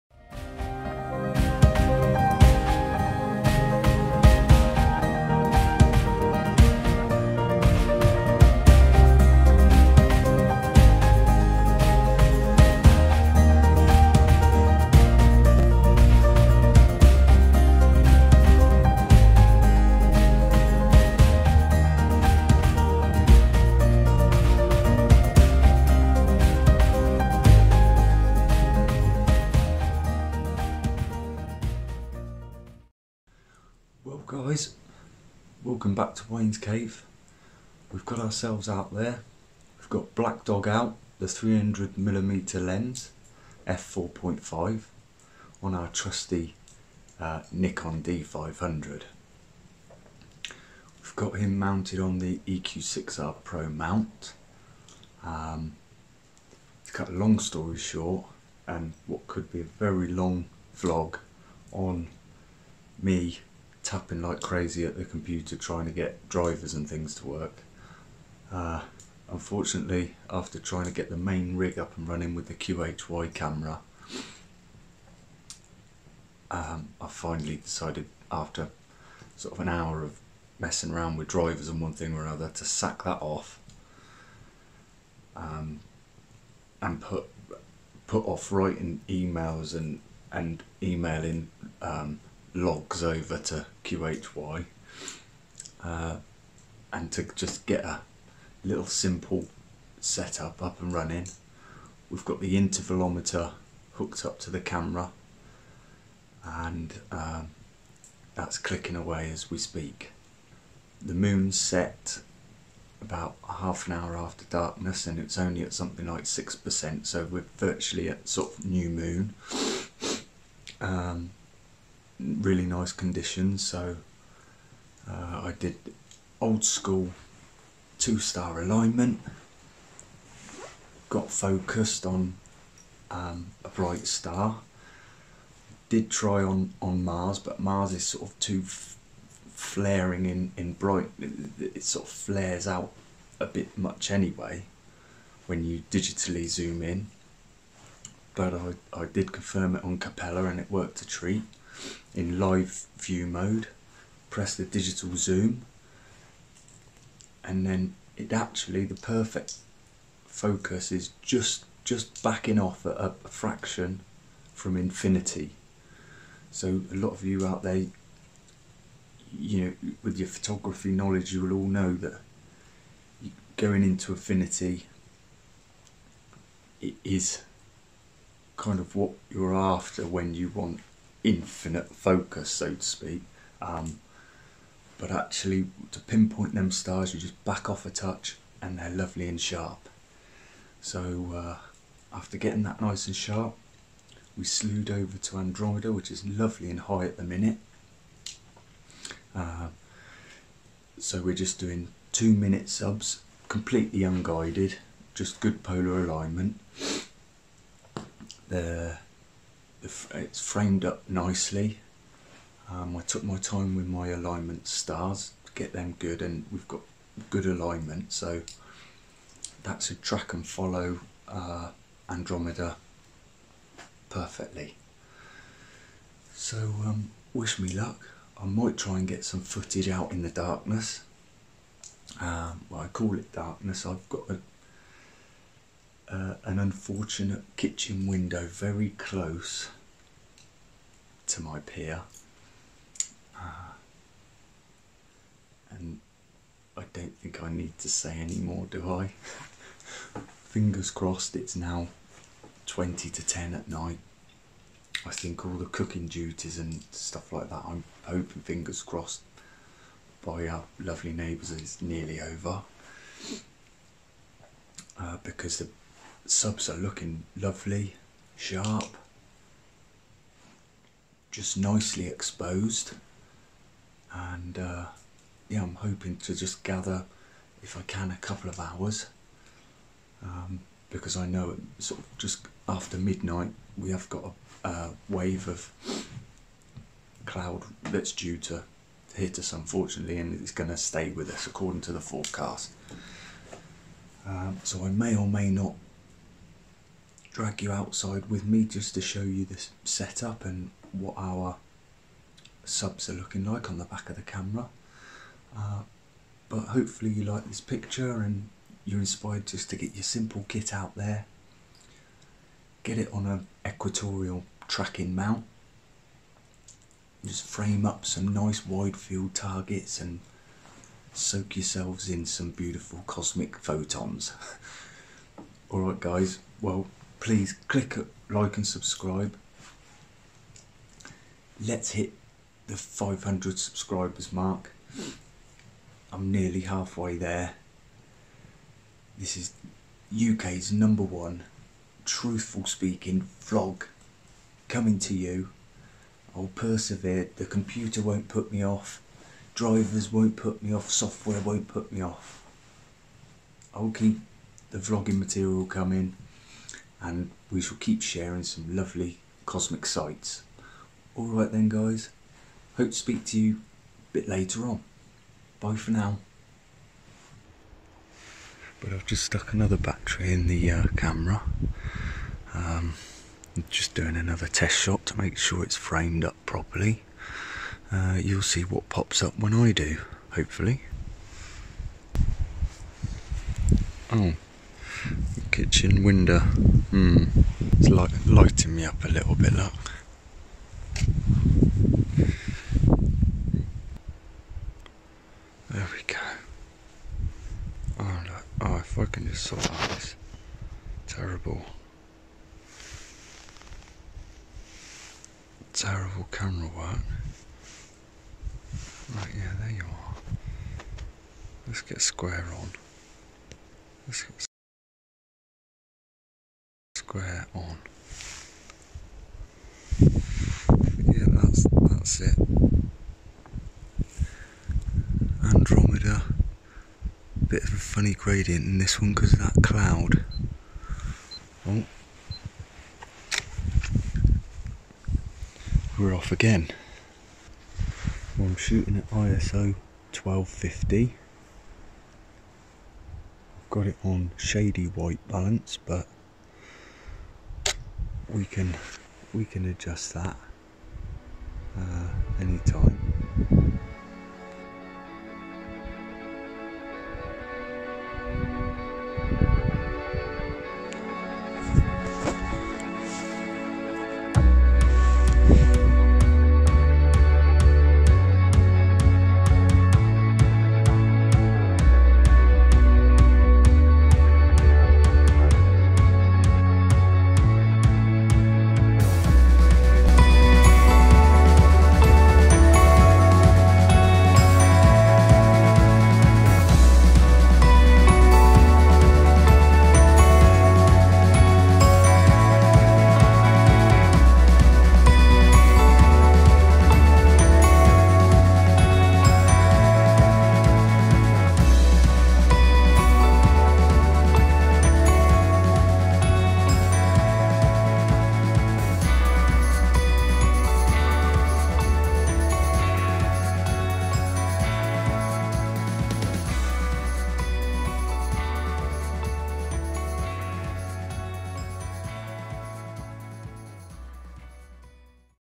DSLR Astrophotography with "Black Dog" - Andromeda Galaxy
Older Japanese made Nikon lenses are affordable in the used market, and although auto focus and stabilization isn't in built,  astrophotography can take advantage of their quality optics!
I use a 1980's 300mm Nikon nikkor lens with my Nikon D500 dslr crop sensor camera, to capture images of the Andromeda galaxy.